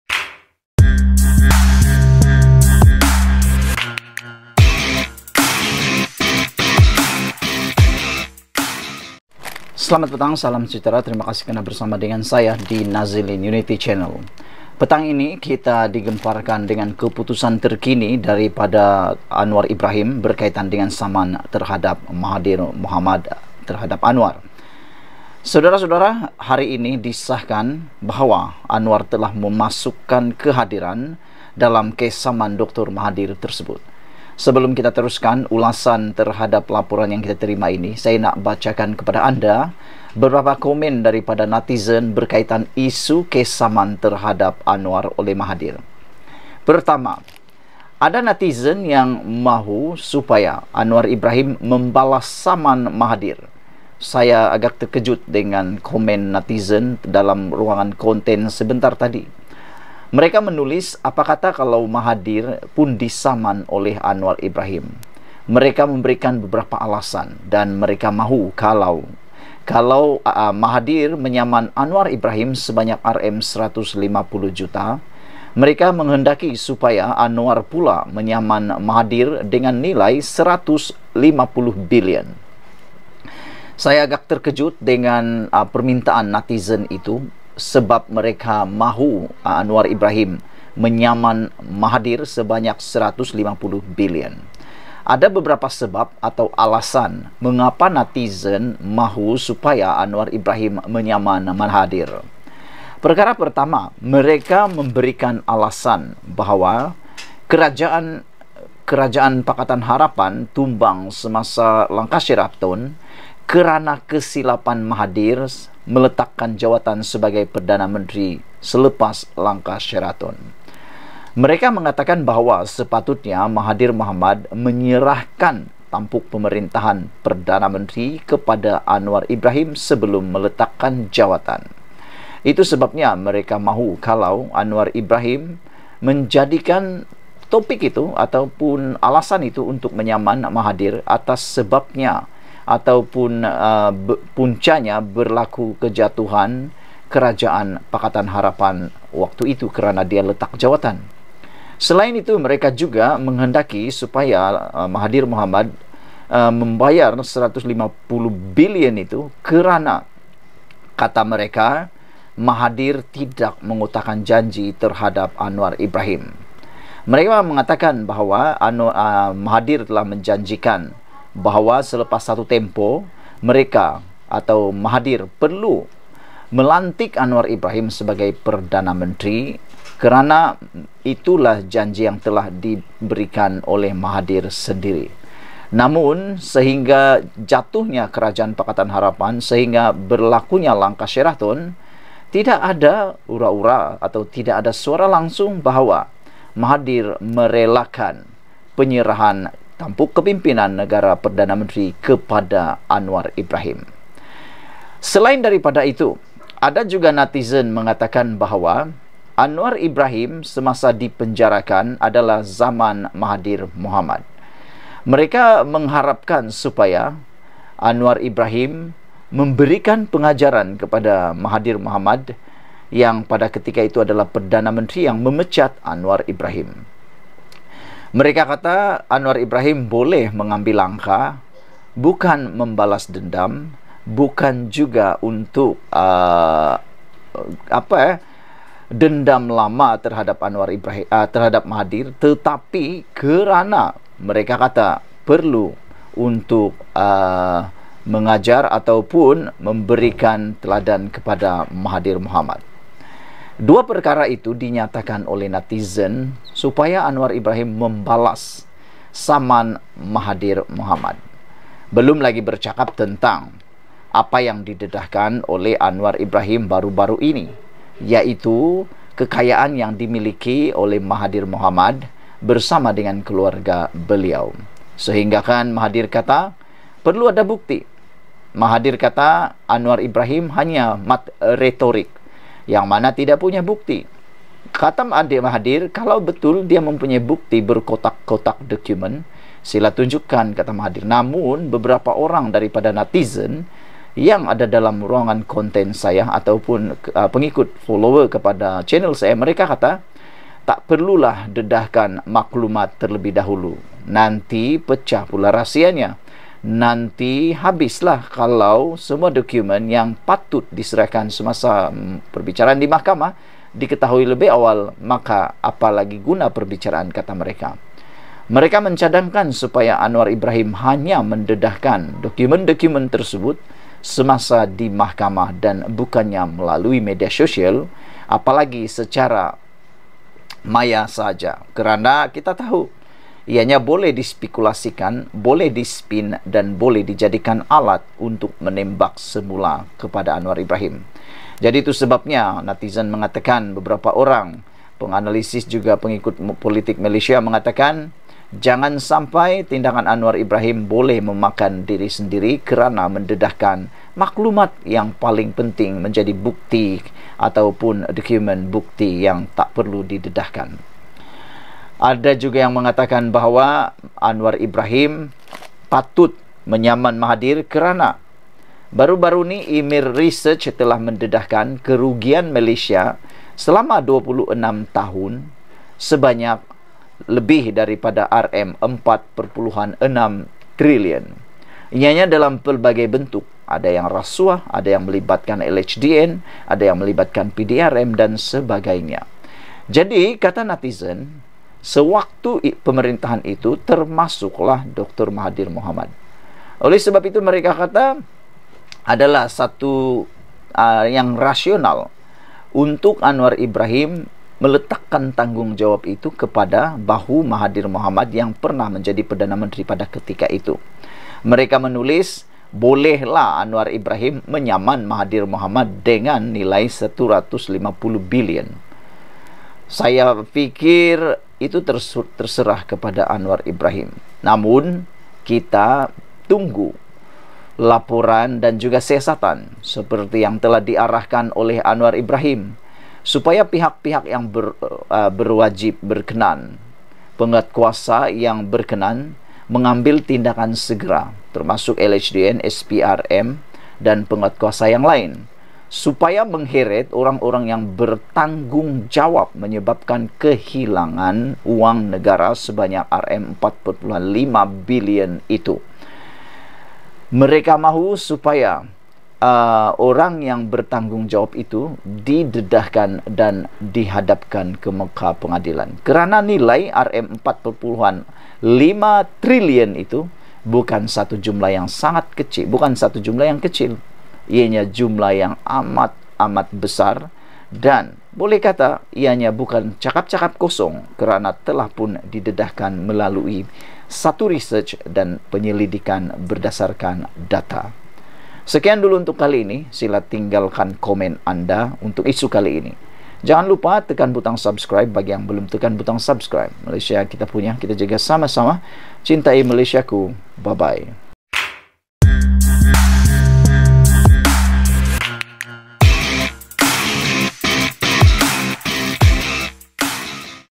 Selepas negara hilang RM4.5 trilion & Harta kekayaan Berbilion, ANWAR SAMAN MAHATHIR RM150 BILLION?
Nazilinunity

Malaysiakini